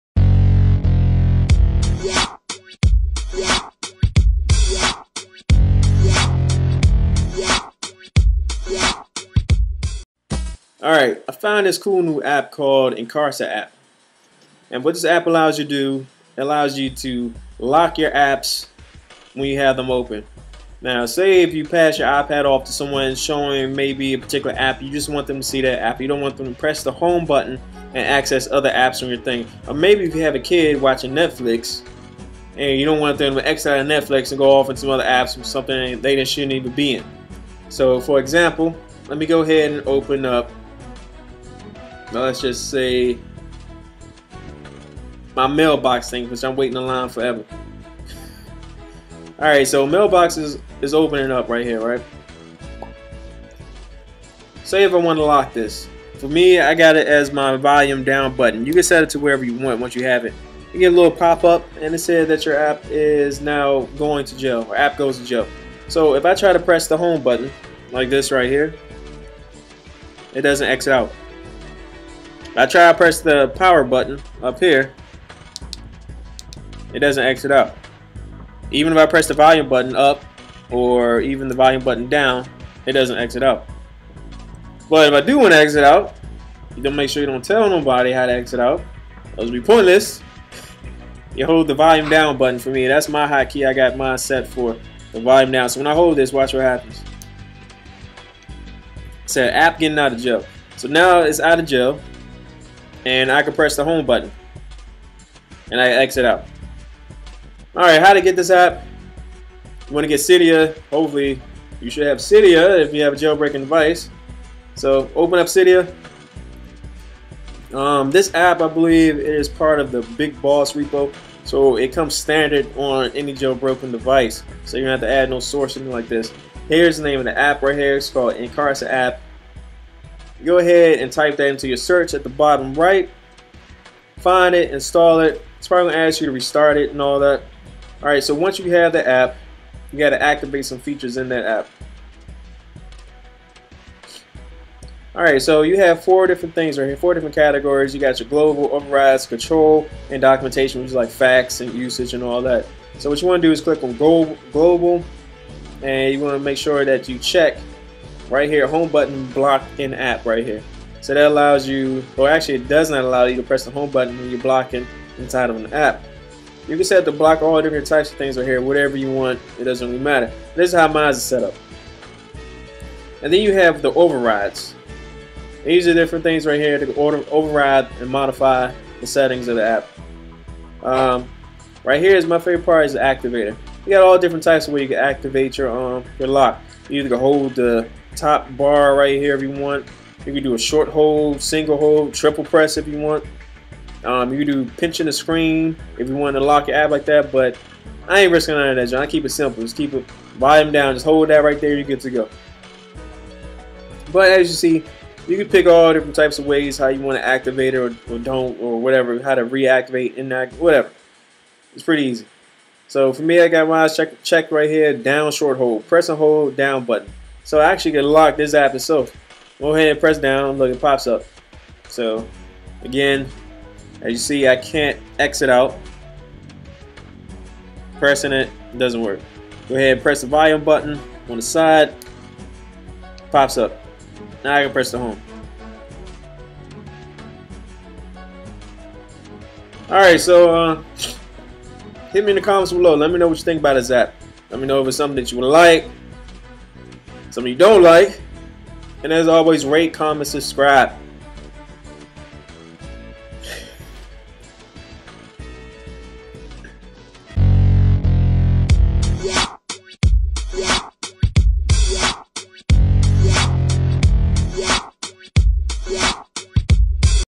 HOw IncarcerApp "Locks Up" Current App, HD Review For iPad
IncarcerApp lets you "lock in" while you are in any iOS app. This is a tweak that you iPhone Monstas who happen to be parents will probably love. Say you want to put on a video for your kid, or let them play in the Crayola app. Chances are they're going to pounce on that home button at some point, and being able to temporarily disable it with IncarcerApp changes all of that.

Vist Our T-Shirt Website Enjoy.